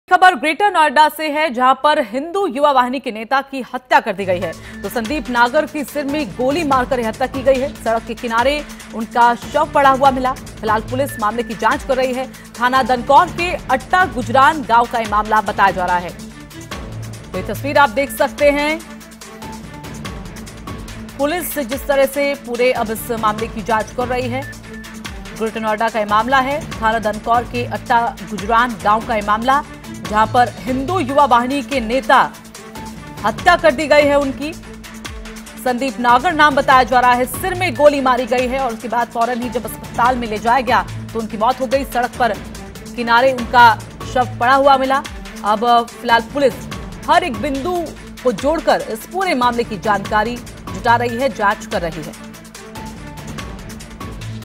हिंदू युवा वाहिनी के नेता का सड़क के किनारे मिला शव
संदीप नागर की सिर में गोली मारकर की गई हत्या...सड़क के किनारे पड़ा मिला शव...पुलिस कर रही मामले की जांच...थाना दनकौर के अट्टा गुजरान गांव का मामला